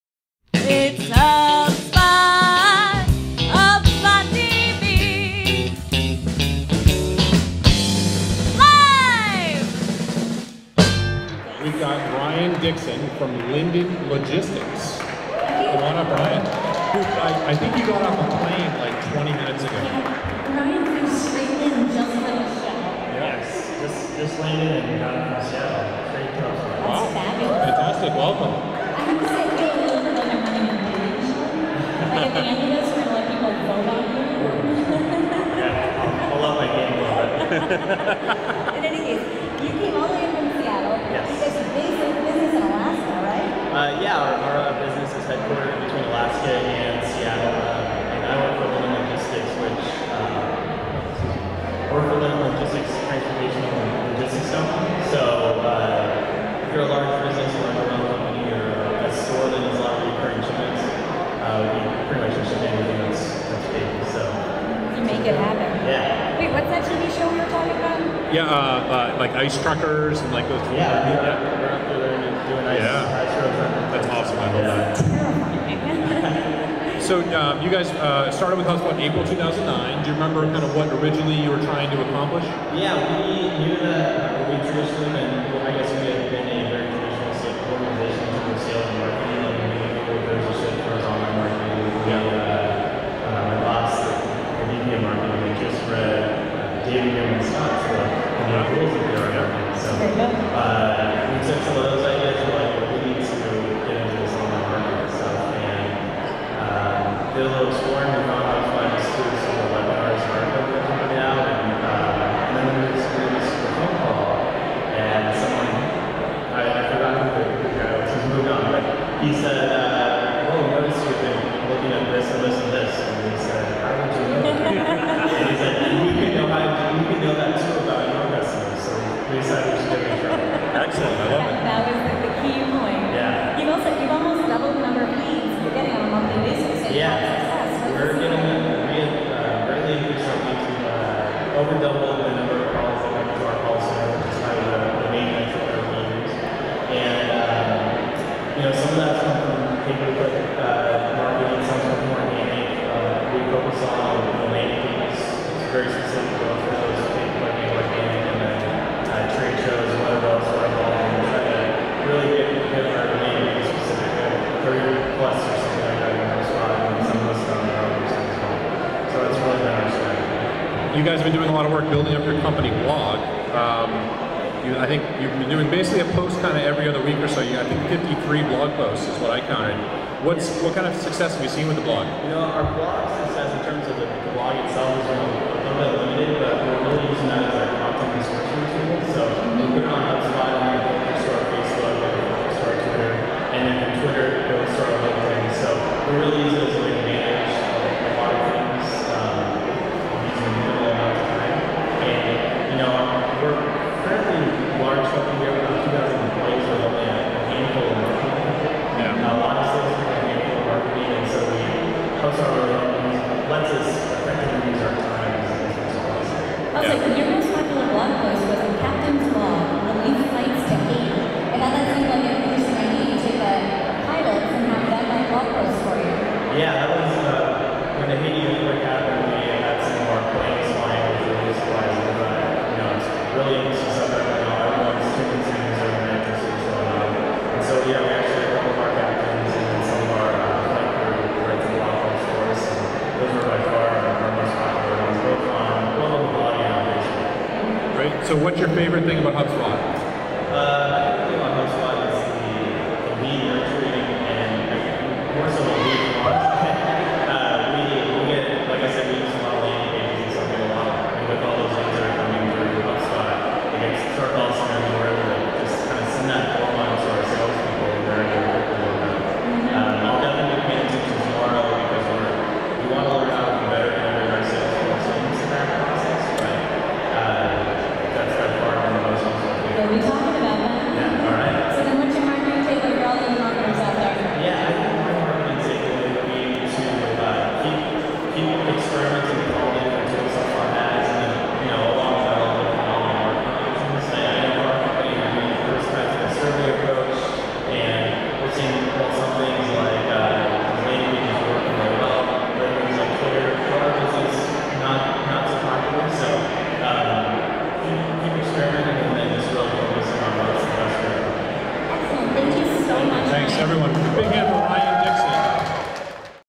HubSpot TV: Ryan Dixon live at HUG 2010
Tell us who you are and what your business is.
You have been using HubSpot since April of 2009, do you remember what problem you were trying to solve when you bought?
Why they bought: SEO 101 best practices, KWG, landing pages, blogging.
You have almost doubled the number of leads you are getting on a monthly basis, what has been your secret?
You also have been actively building up your company blog. What successes have you see there?
53 blog posts - 1 every other week
Most Popular Blog Post: Captain's Blog: Relief flights to Haiti - Captain Chris Caden shares his experiences
What is your favorite thing about HubSpot?
What is your Marketing Takeaway for all the viewers out there?